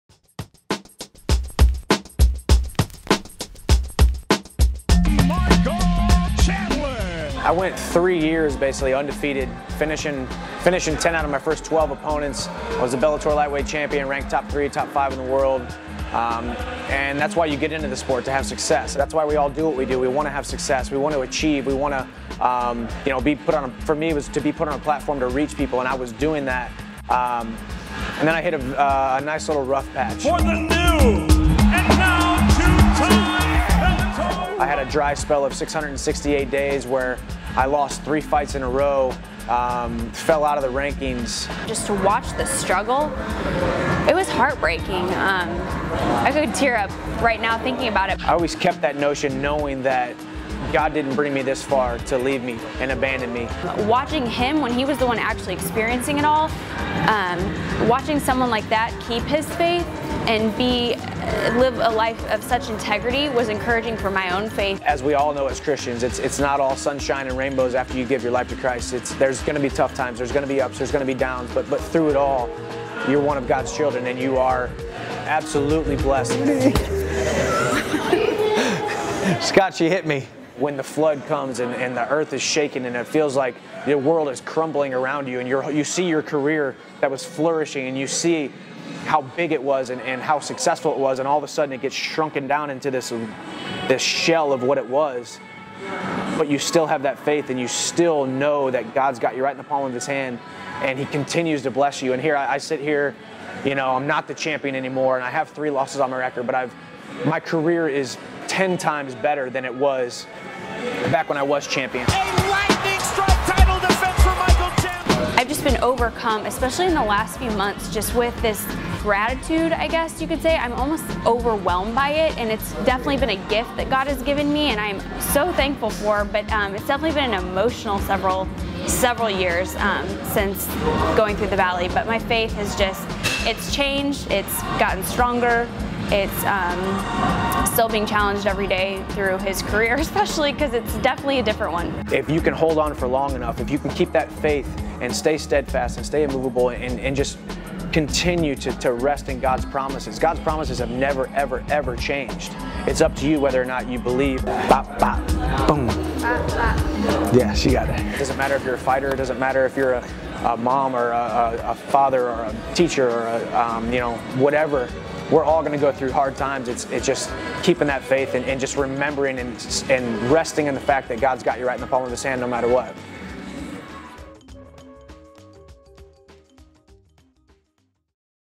Interview with Michael Chandler
MMA Bellator Champion Michael Chandler and his wife Brie sits down with Outreach Pastor Scott Laughlin to discuss how faith fits into his role and influence as one of the greatest MMA Athletes of today!